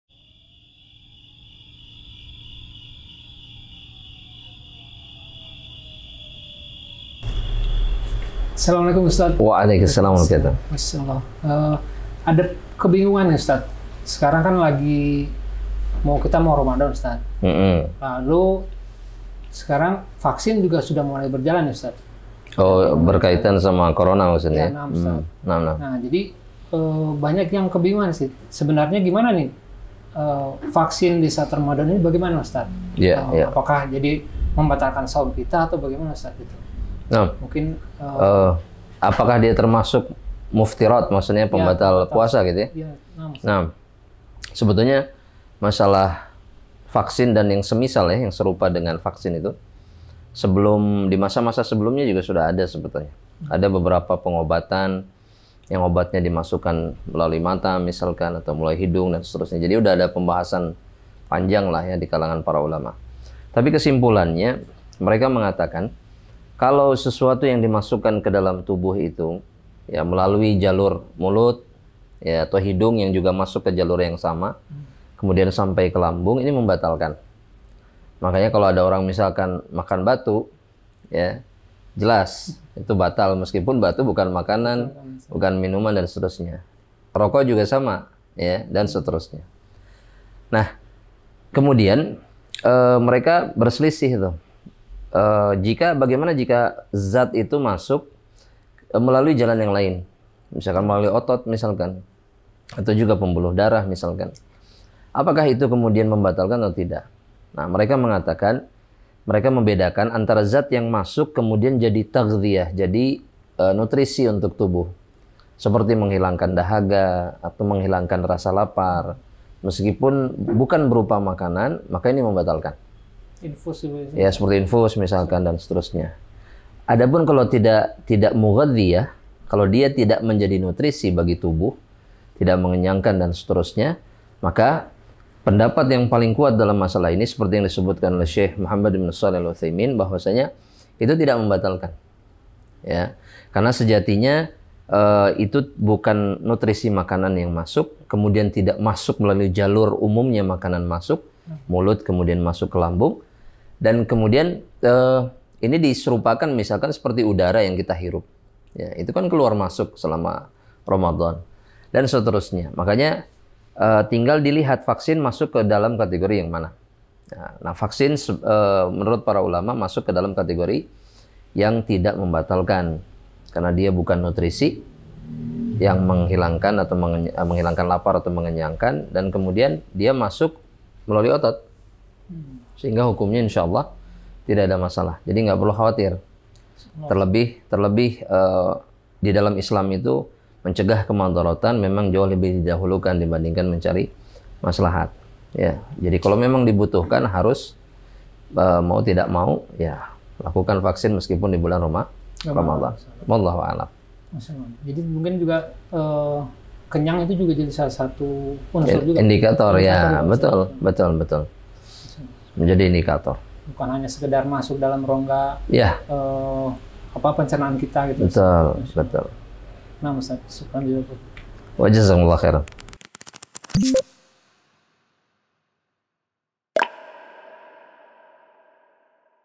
Vaksin Membatalkan Shaum?
Apakah Vaksin Membatalkan Shaum | Ustadz Abdullah bin Amir Maretan Hafizhahullah
Di masa pandemi seperti ini mungkin sebagian dari kita ada yang akan menjalani vaksin di bulan ramadhan ini, apakah menjalaninya akan membatalkan shaum kita, yuk kita simak penjelasan dari ustadz shob